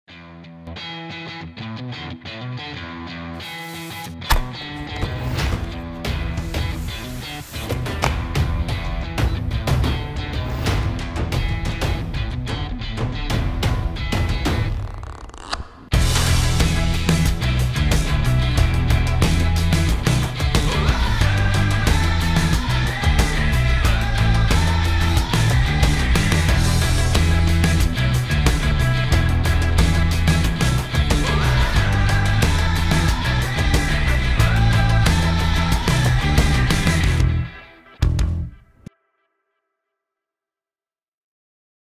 Unboxing Zhiyun Crane 3S (2020)
Presentación del Crane 3S, un poderoso gimbal de la marca Zhiyun diseñado para los más ambiciosos, con un soporte para hasta 14.5lbs. En definitiva, una bestia!